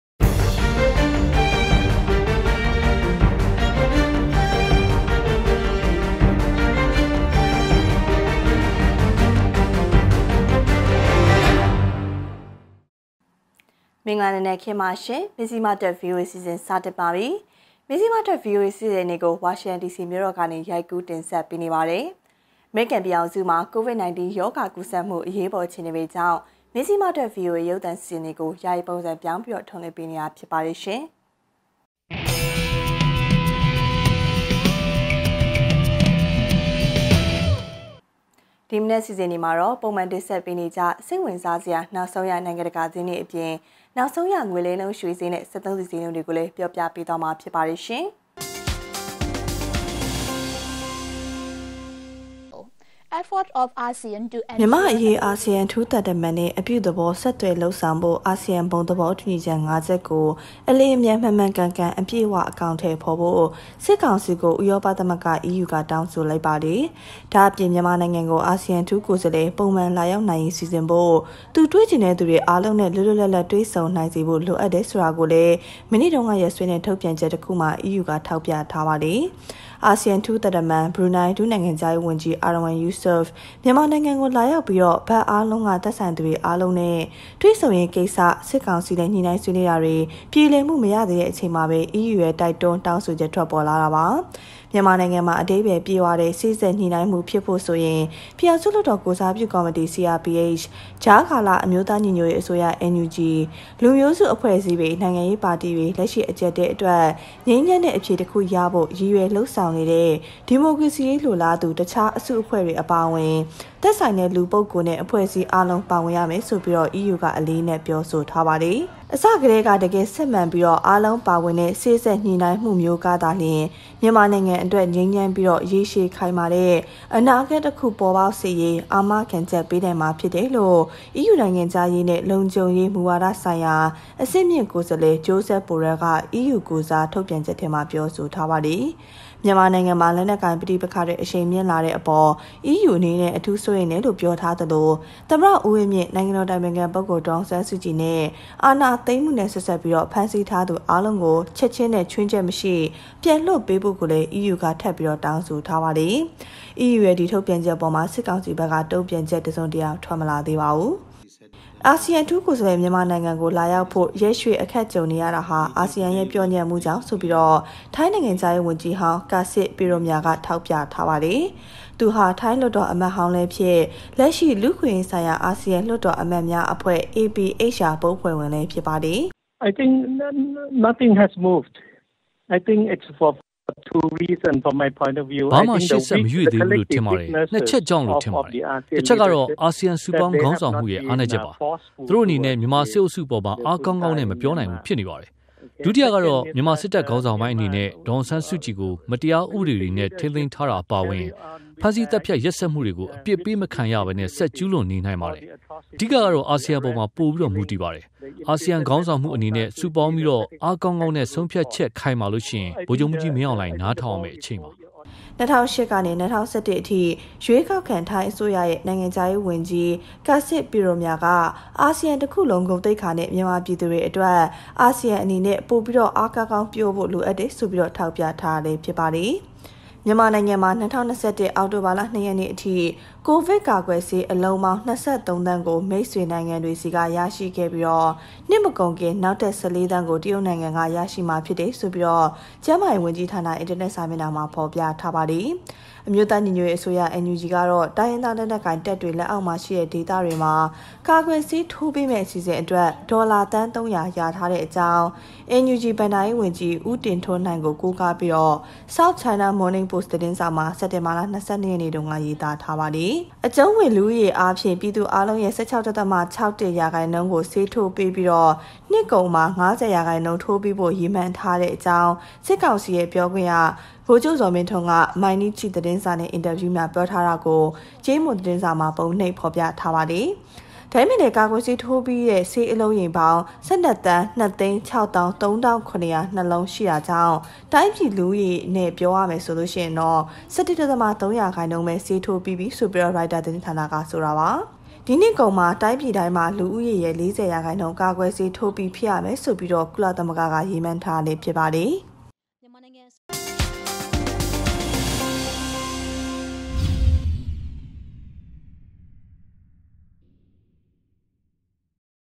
အာဆီယံဘုံသဘောတူညီချက် ၅ ချက် အကောင်အထည်ဖော်ဖို့ EU တောင်းဆို
မြန်မာ့အရေး အာဆီယံအထူးသံတမန်နဲ့ အပြုသဘော ဆက်သွယ်လုပ်ဆောင်ဖို့၊ အာဆီယံ ဘုံသဘောတူညီချက် ငါးချက်ကို အလျင်အမြန် မှန်မှန်ကန်ကန် အပြည့်အဝ အကောင်အထည်ဖော်ဖို့ စစ်ကောင်စီကို ဥရောပသမဂ္ဂ (EU) က တောင်းဆိုလိုက်ပါတယ်။ အာဆီယံ အထူးသံတမန် မြန်မာနိုင်ငံကို လာဖို့ ရက်ရွှေ့အခက်ကြုံနေရတာဟာ အာဆီယံရဲ့ ပျော့ညံ့မှုကြောင့်လို့ထိုင်းနိုင်ငံခြားရေးဝန်ကြီးဟောင်း Kasit Piromya က ထောက်ပြပါတယ်။ ဒီလို မြန်မာနိုင်ငံဆိုင်ရာ သတင်းတိုတွေကို မအိသန့်စင်က တင်ဆက်ထားပါတယ်။